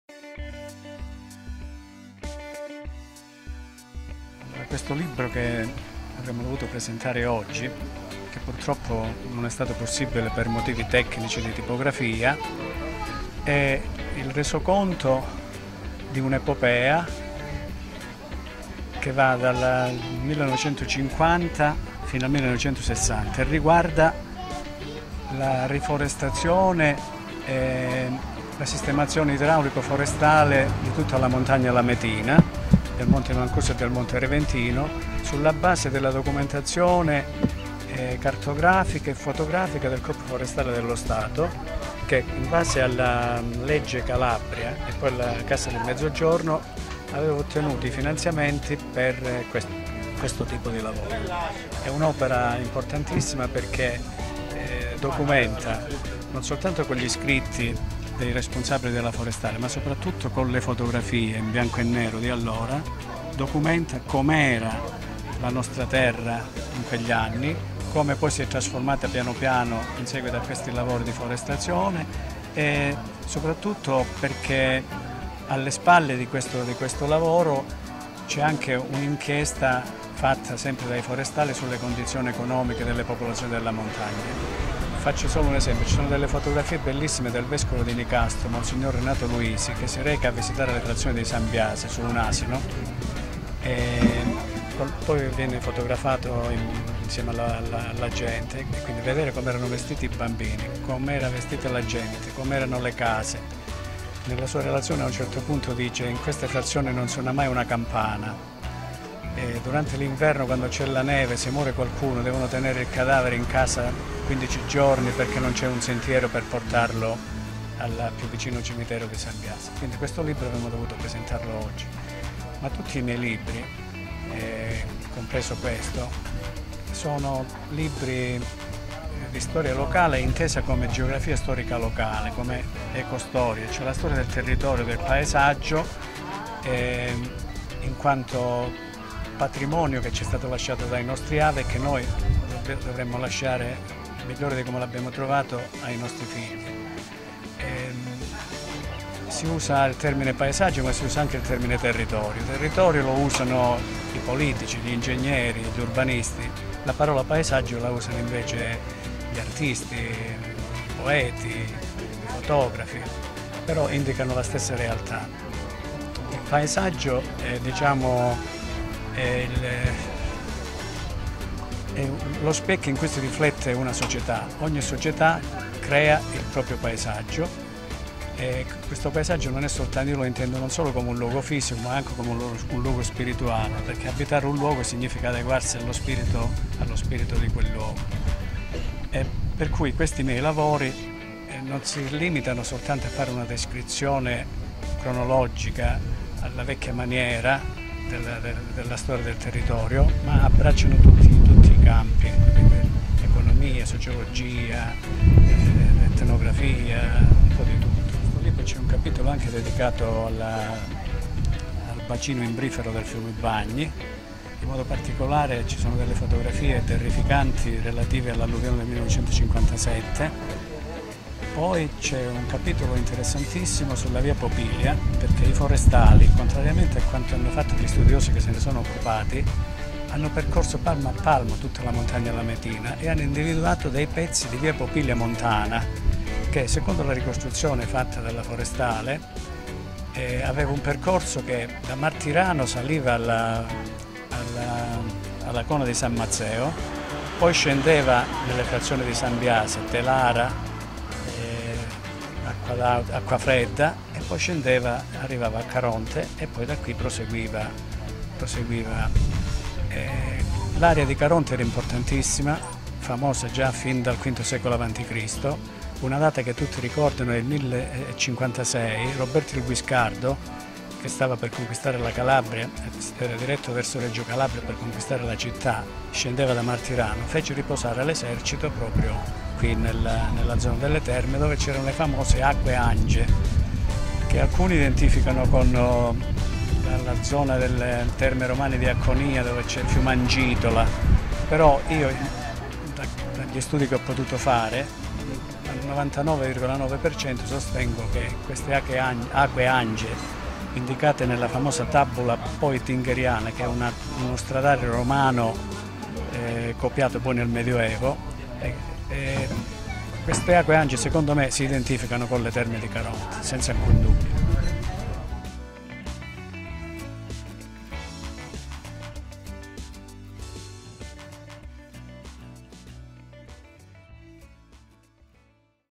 Intervista al Prof. Vincenzo Villella
Registrazione effettuata da liberi.tv il 14 luglio 2019

Intervista a Vincenzo Villella, pubblicista, docente e scrittore.
Lamezia Terme (Cz)